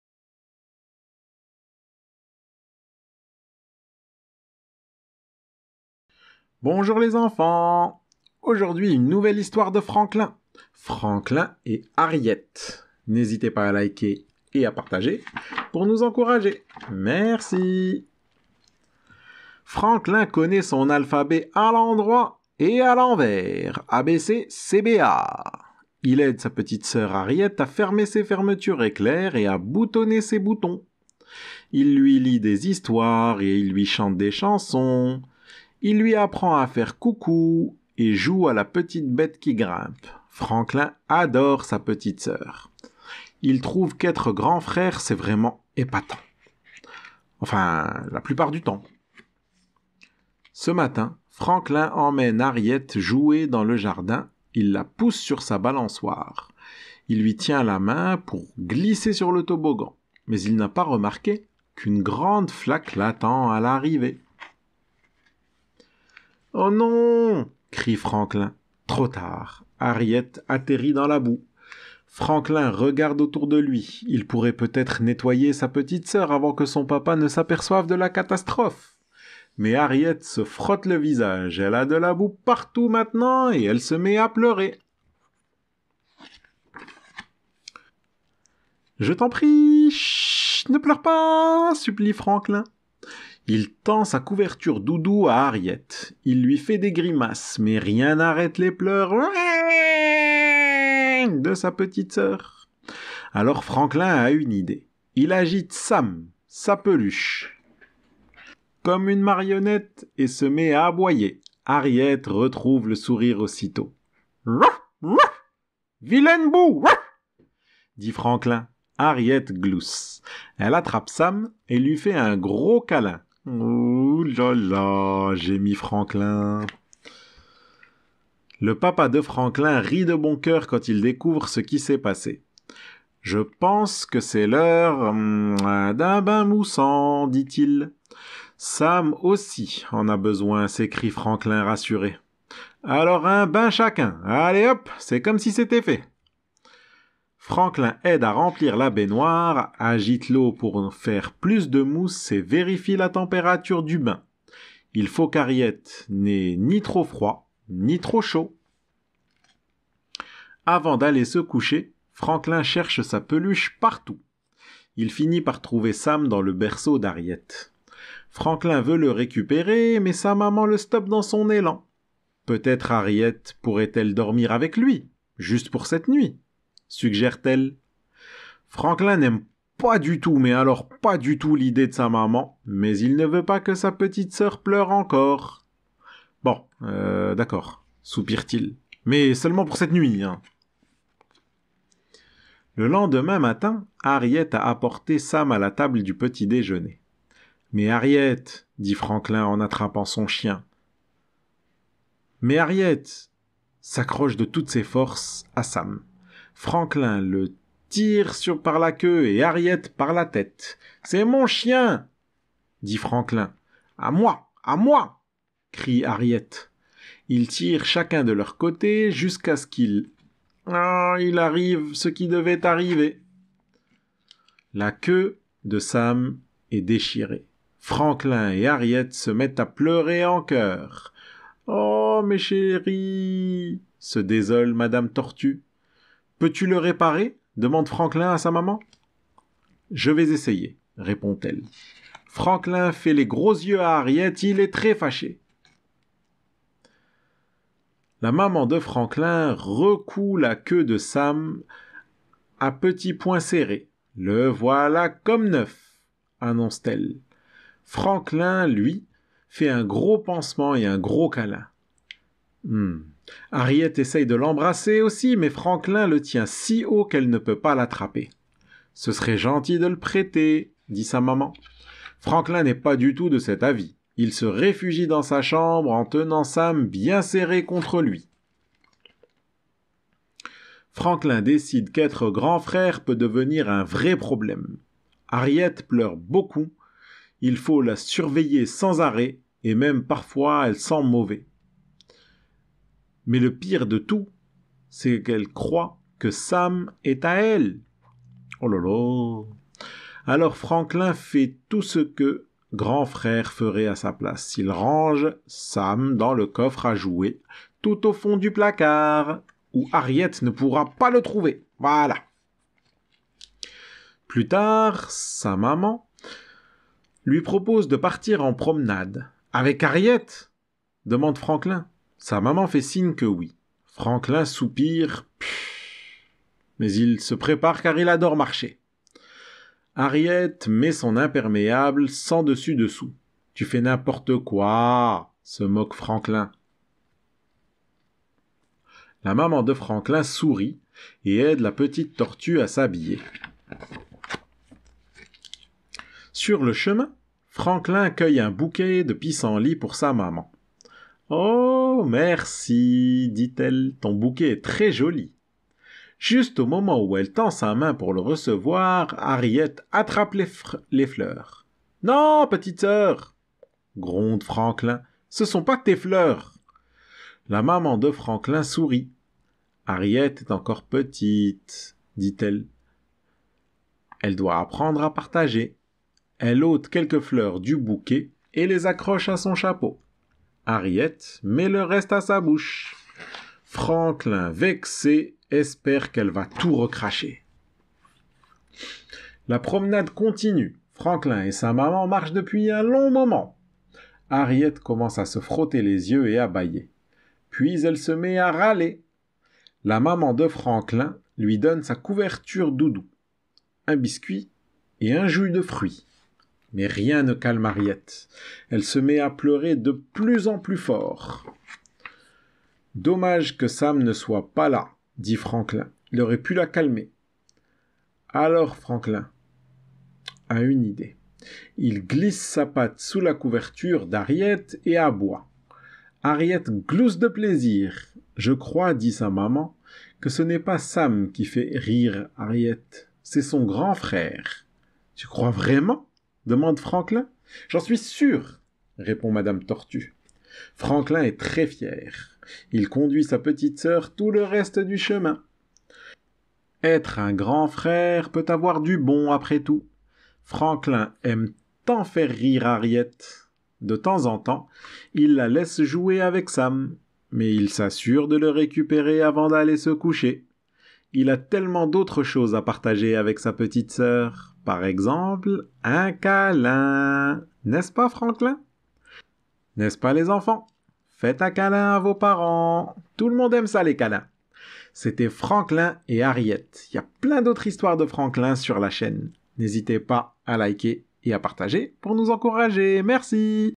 Lecture Pour S'endormir : Franklin Et Harriet
Retrouvez ici tous les jours plein de lectures d'albums jeunesse et de livres enfants

Retrouvez Franklin dans une histoire toute mignonne avec sa merveilleuse petit soeur Harriet.

Autrices:  Paulette Bourgeois (Auteur) Brenda Clark (Illustration)
Éditions : DEUX COQS D'OR